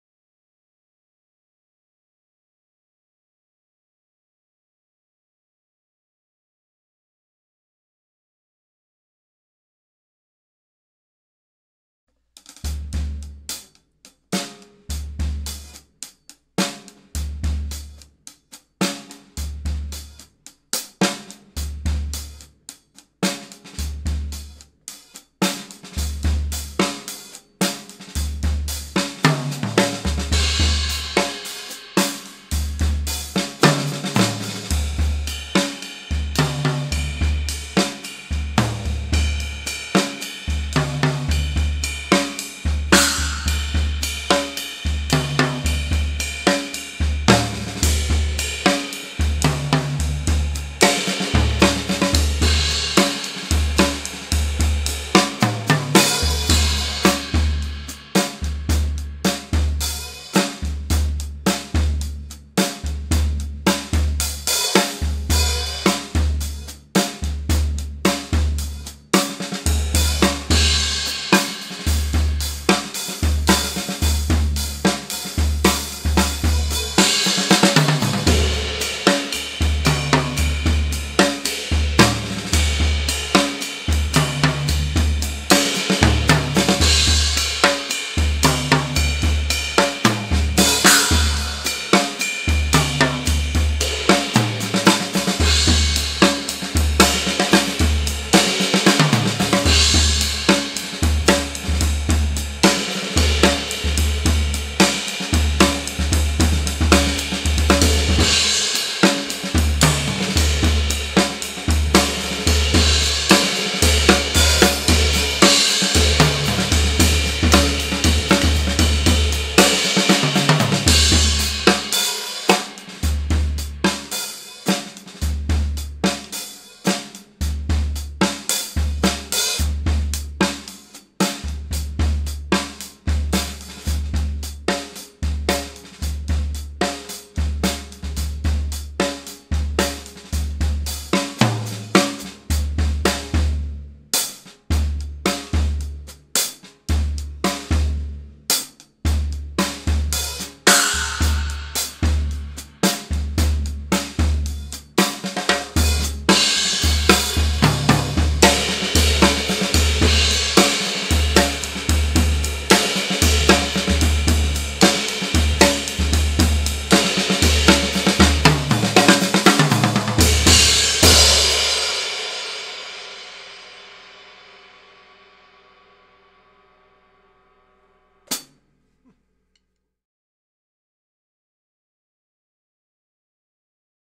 Jerome Deupree Plays His Sonor Phonics - Part 3
Gear list: Sonor Phonic 14" x 18" bass drum, 8" x 12" rack tom, 14" x 14" floor tom, and 5.75" x 14" Ferro snare drum; vintage A. Zildjian 14" Hats, Paiste Dark Energy 10" Prototype Splash over Sabian HHX 17" Legacy Crash, and Paiste Twenty Masters Series 22" Dark Ride; Regal Tip Bob Gatzen Maple sticks.